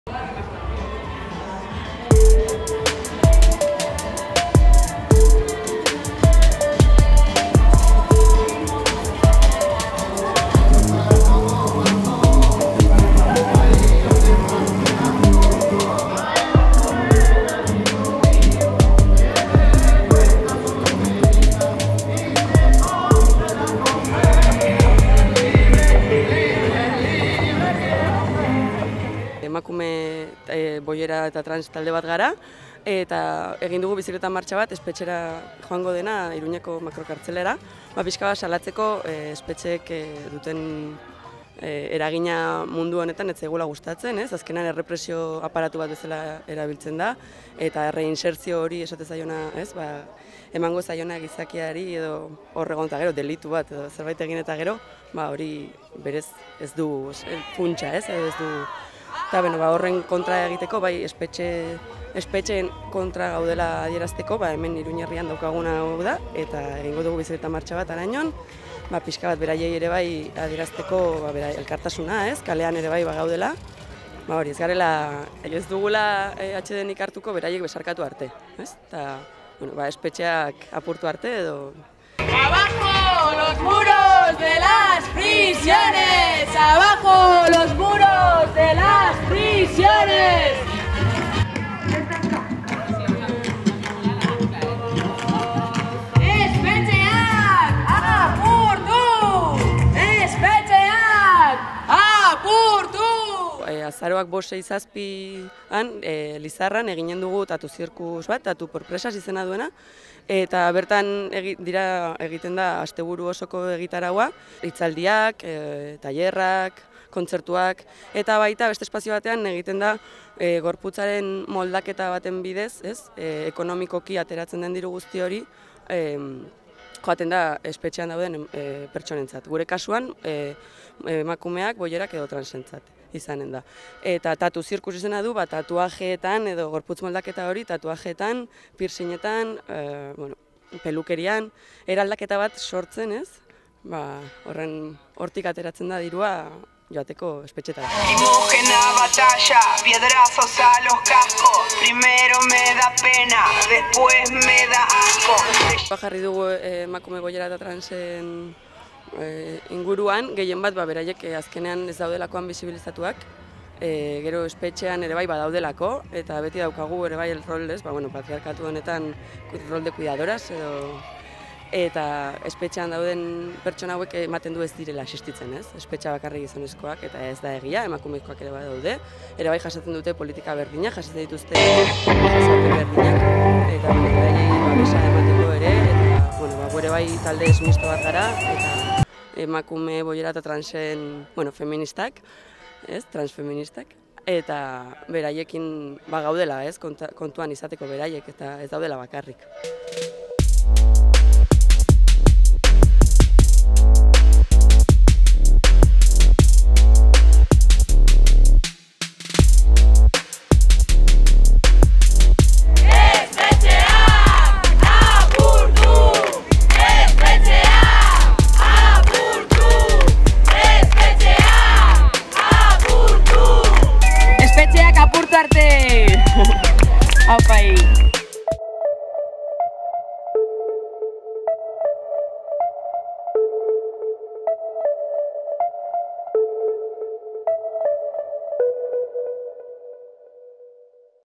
Azaroaren 5, 6 eta 7an espetxeen aurkako topaketak ospatuko dituzte Lizarran
TattooXpresas izeneko ekimena aurkezteko, gaur Iruñeko espetxera txirrindula martxa bat egin dute. Azaroaren 5, 6 eta 7an Lizarrako Sugarra Gaztetxean espetxeen aurkako topaketak eginen dira.